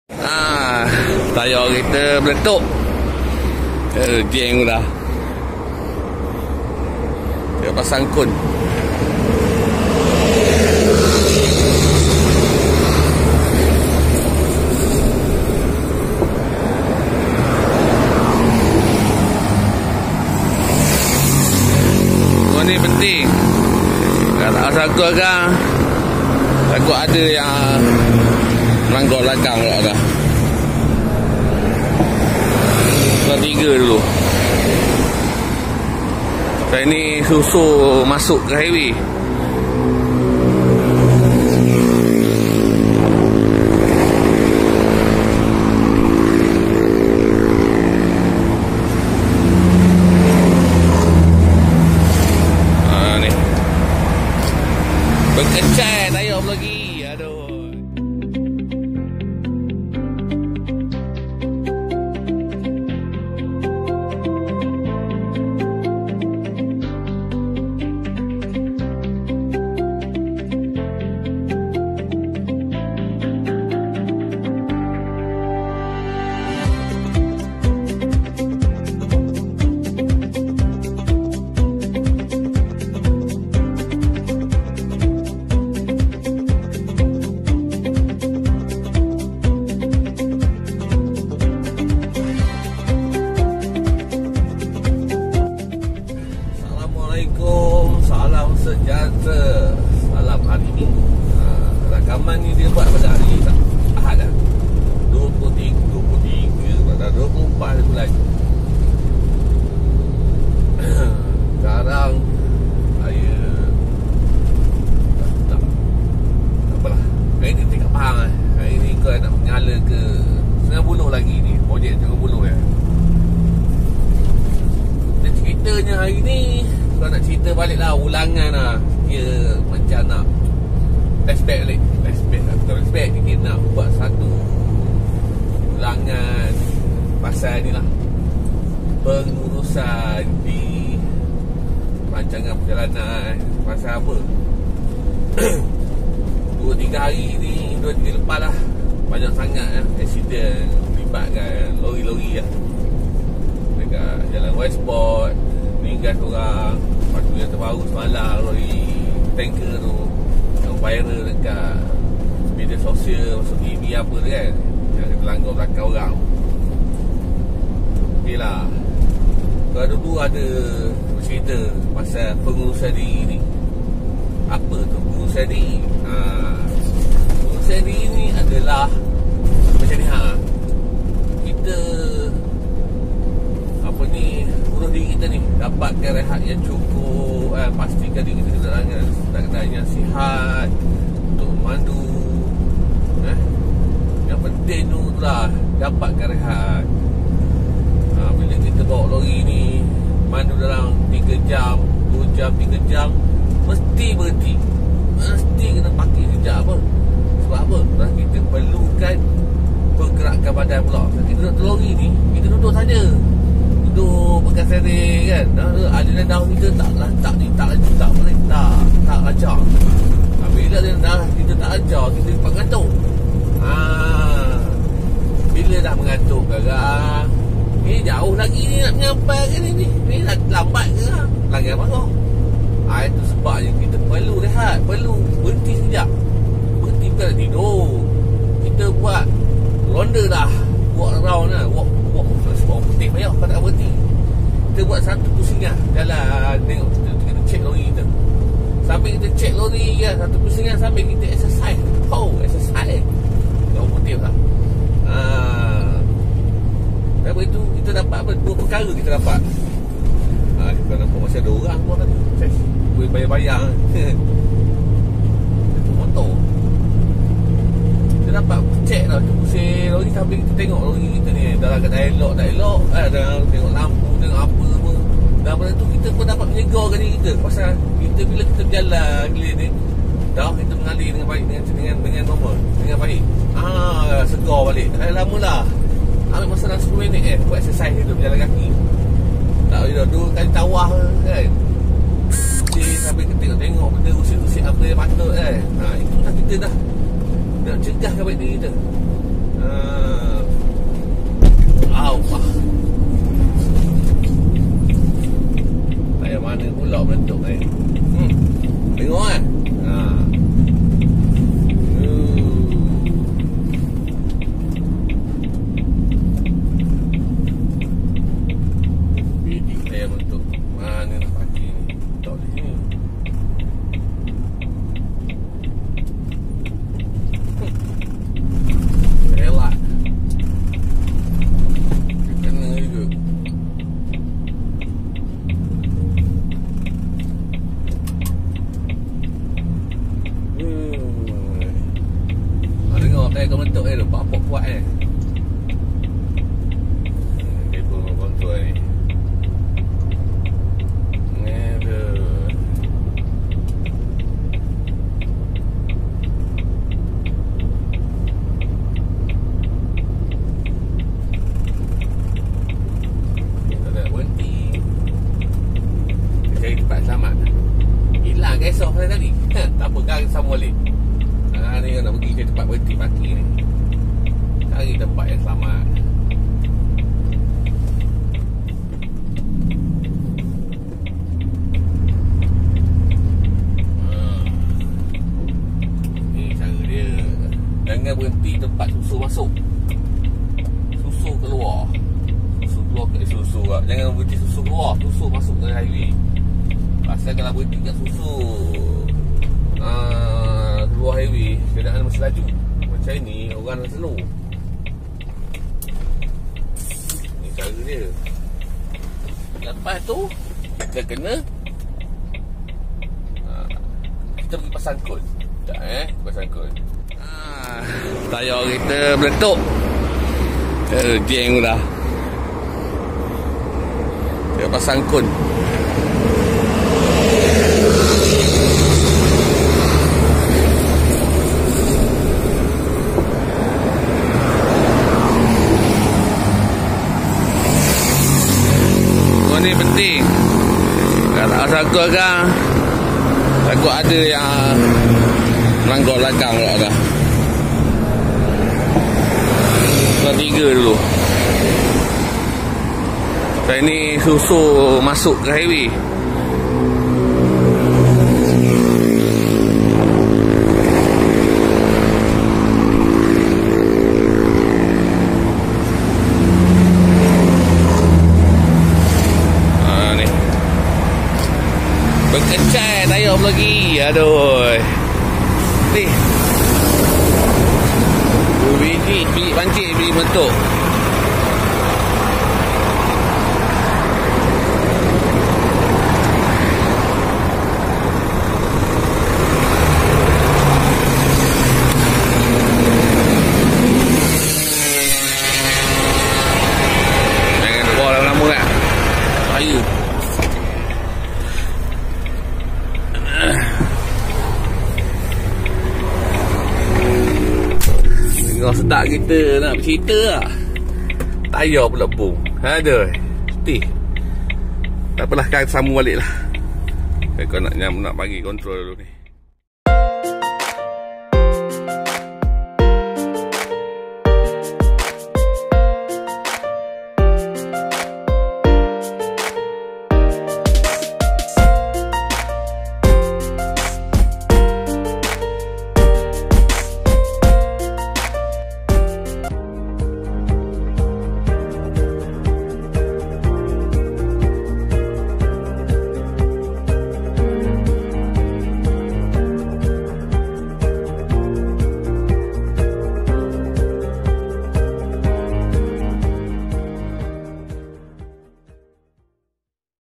MUSIBAH DIHARI MINGGU NKVE DAMANSARA..
Tgh sedap bercerita tetiba tayar meletup aptb. waktu ni la pulak nk pecahnya tayar 😂😂😂